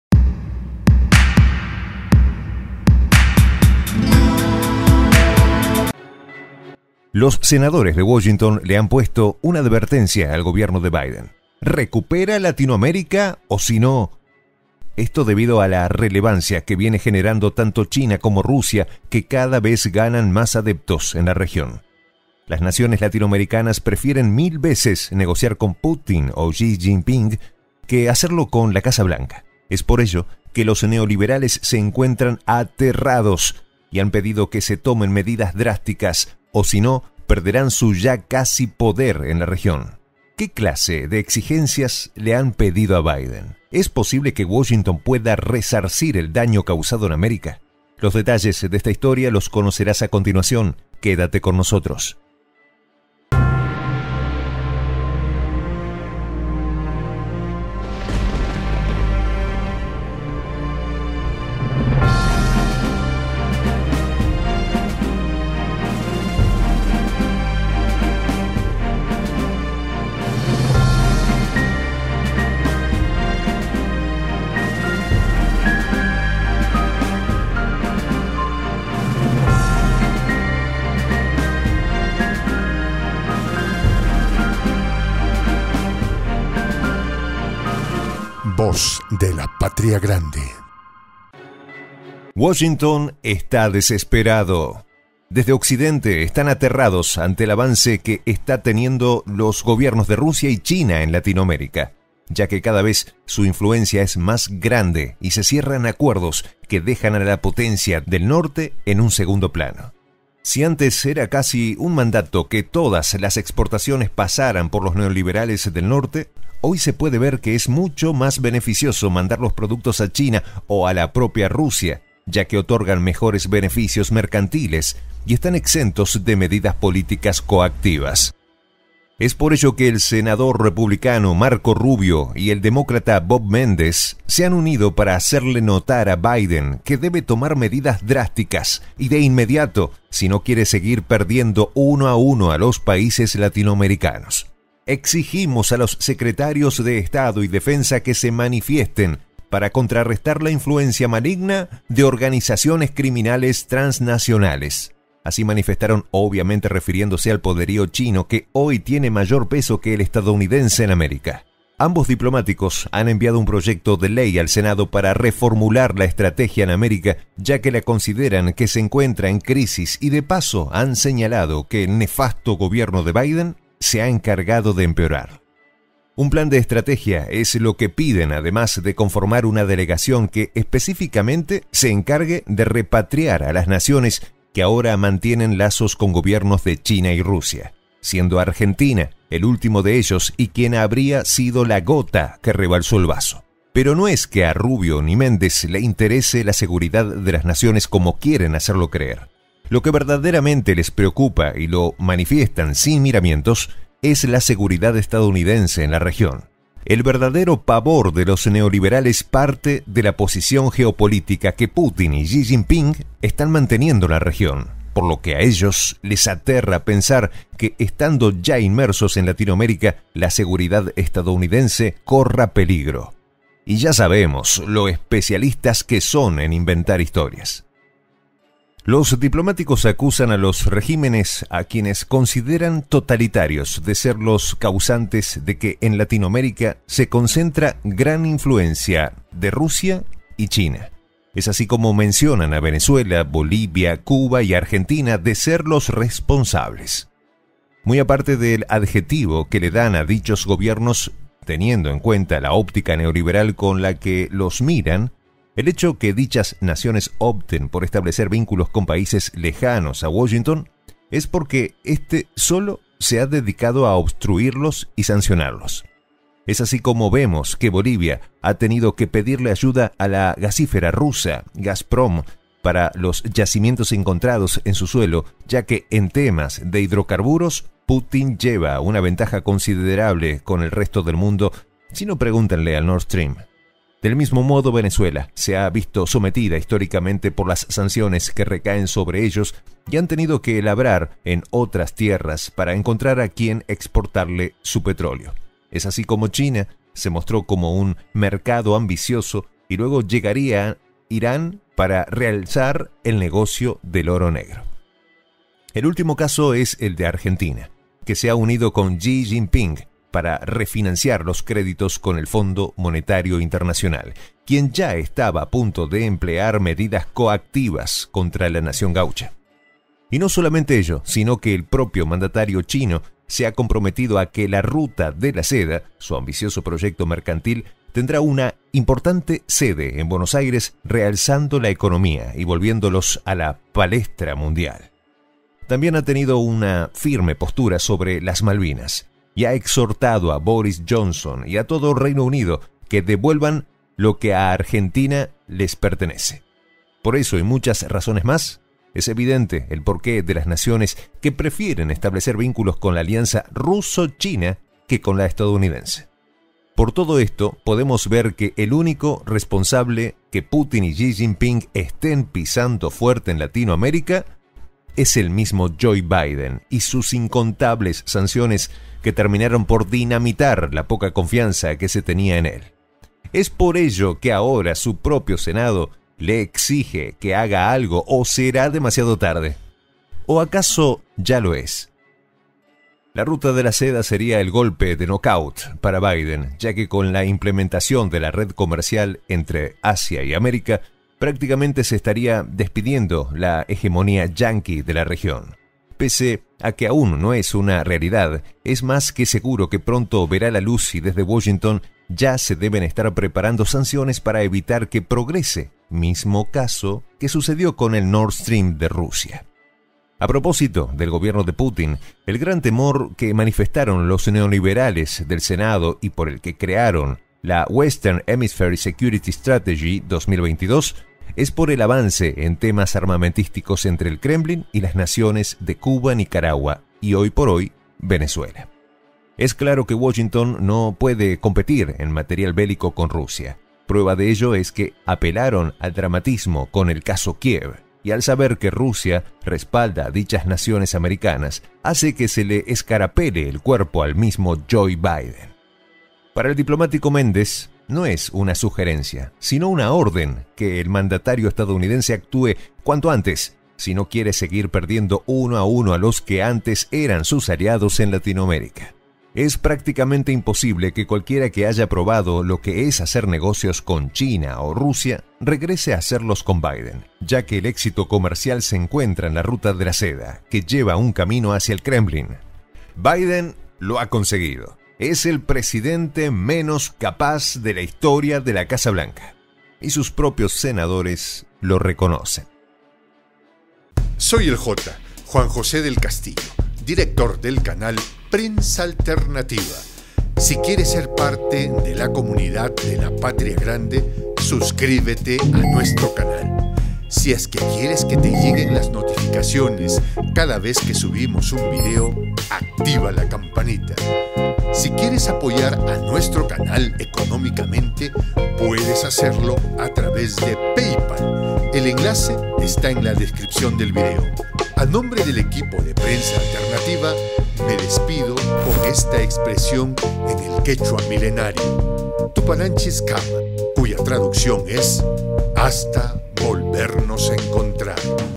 Senado da ultimátum a Biden para recuperar influencia en la Patria Grande!!
Dos importantes diplomáticos estadounidenses han manifestado a Joe Biden la evidente preocupación debido a que cada vez pierden más ‘piso’ en Latinoamérica. En modo imperativo y a modo de advertencia le han dicho a Biden: ¡Recupera Latinoamérica!...

Jefe de Prensa: Enzo Anchante